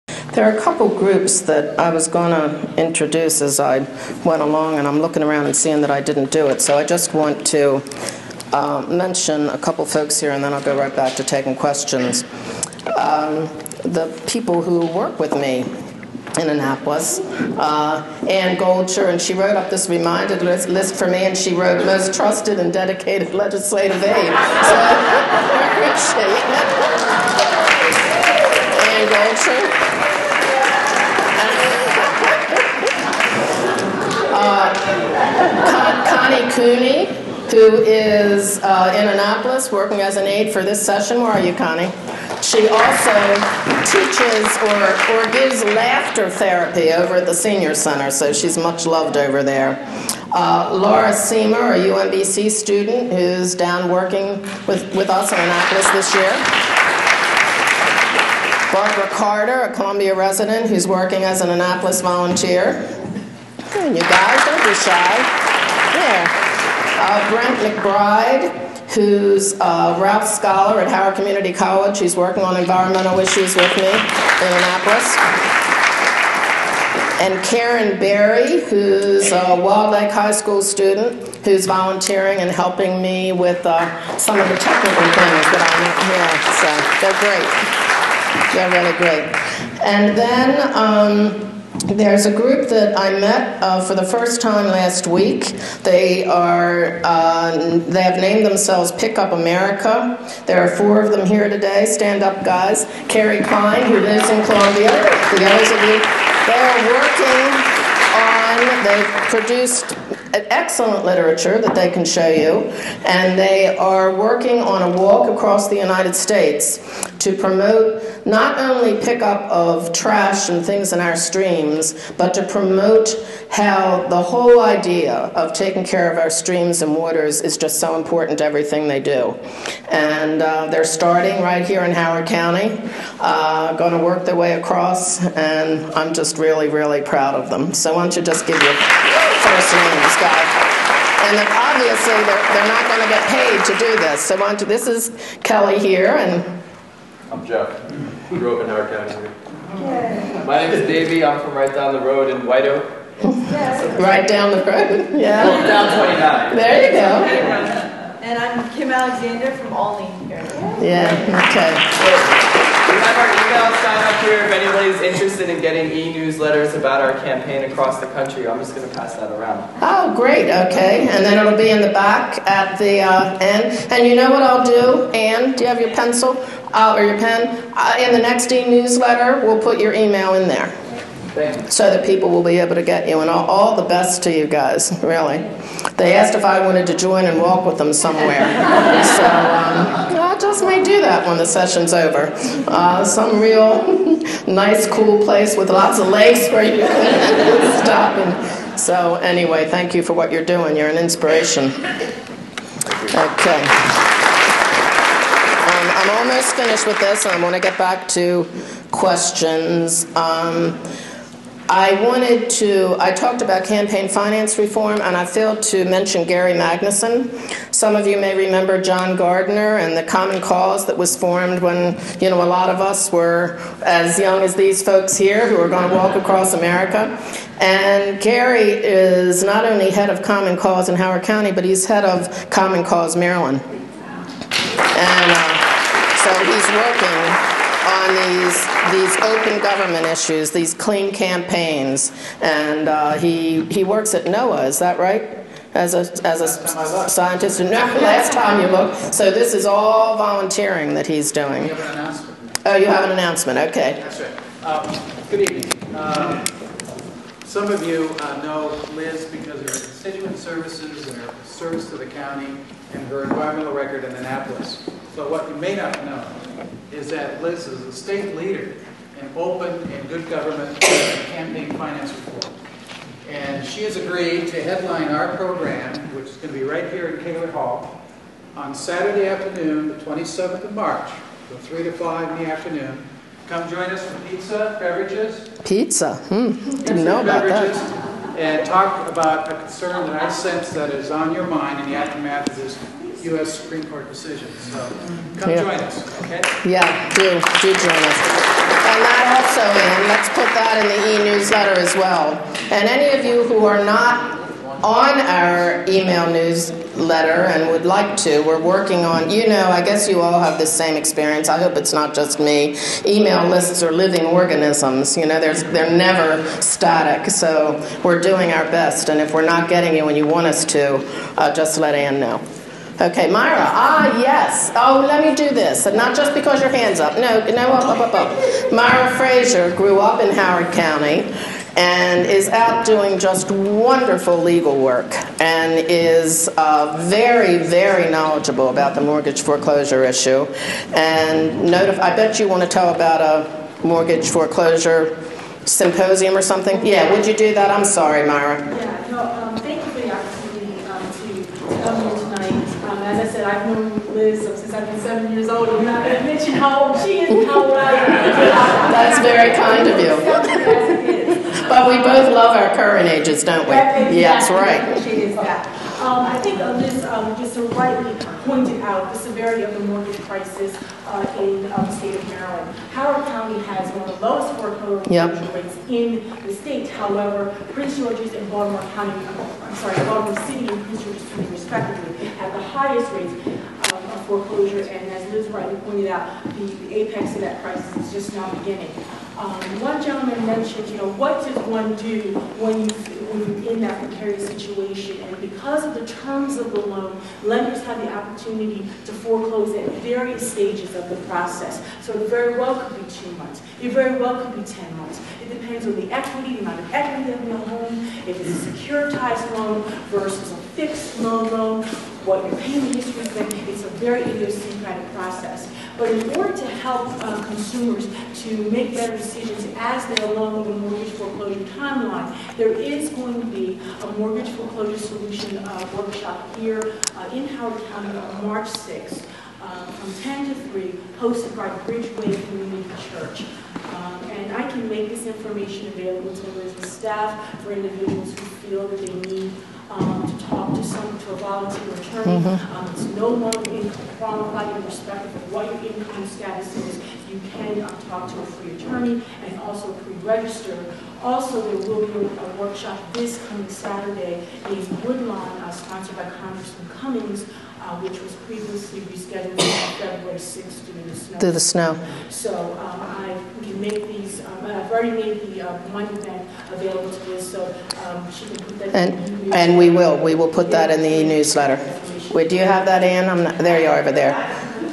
Elizabeth Bobo, MD State Delegate, District 12b - Town Hall Feb 2010 Part 05
HoCoMoJo provides this unedited coverage of Liz Bobo's February 2010 Town Hall community meeting.